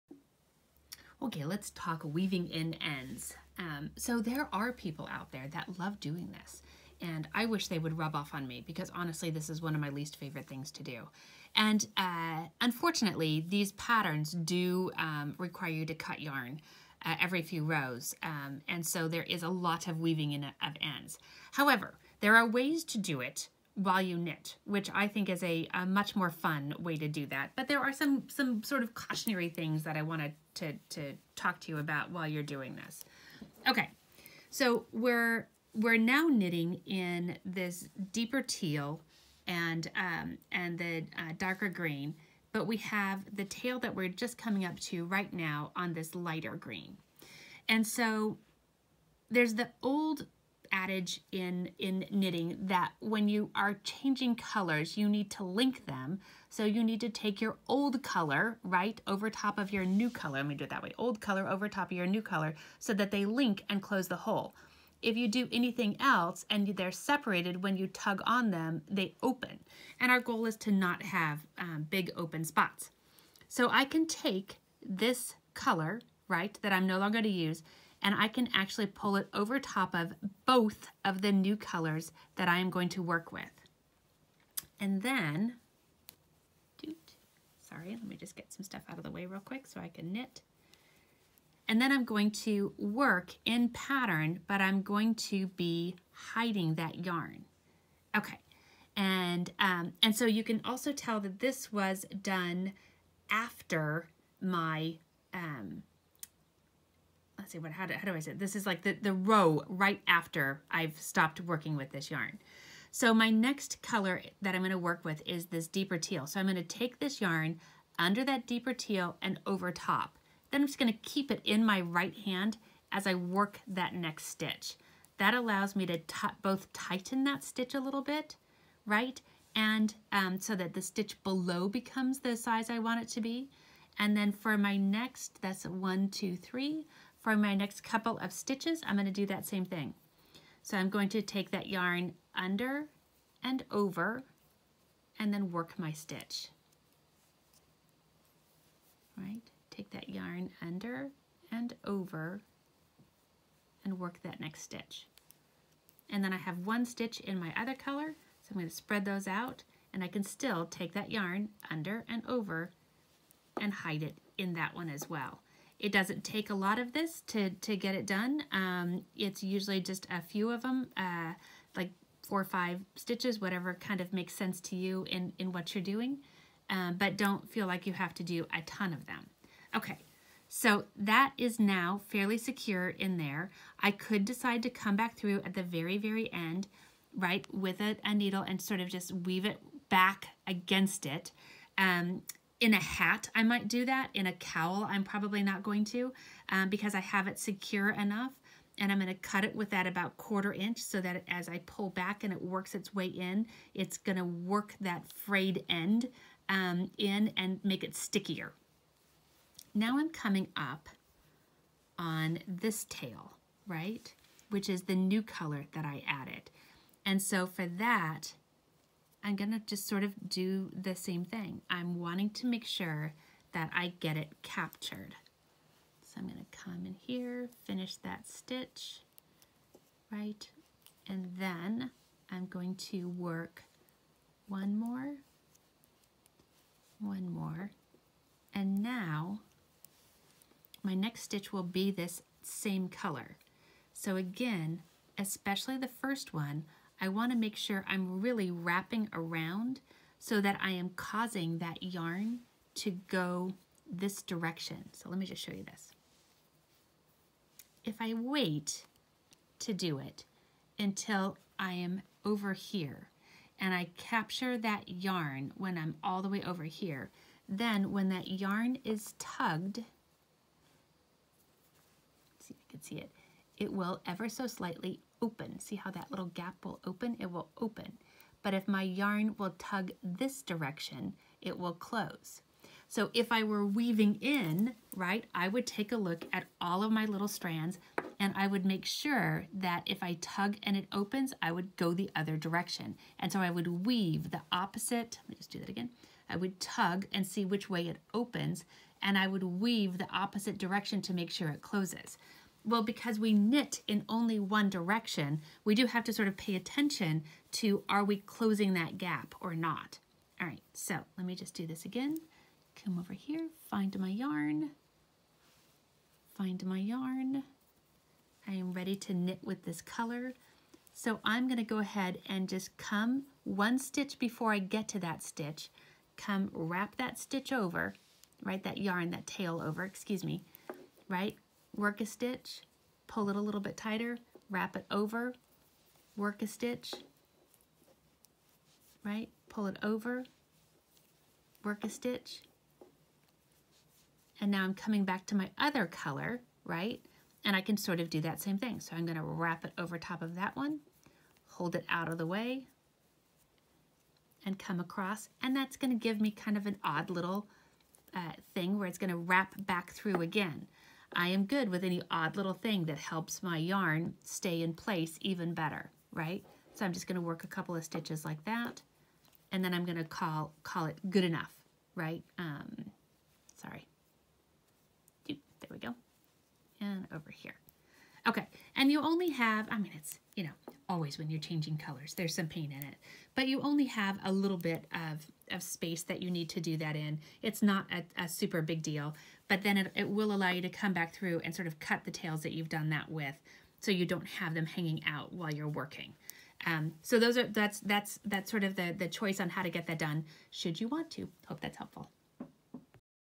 Weaving in Ends as you Knit
This video shows how to weave in your ends on the go.  It trouble shoots some of the pitfalls in how to weave in on the go - especially avoiding holes.

This video was shot to support the LYS Day 2023 pattern, Have it your Way by MollyConroy using a kit created by Alexandra the Art of Yarn.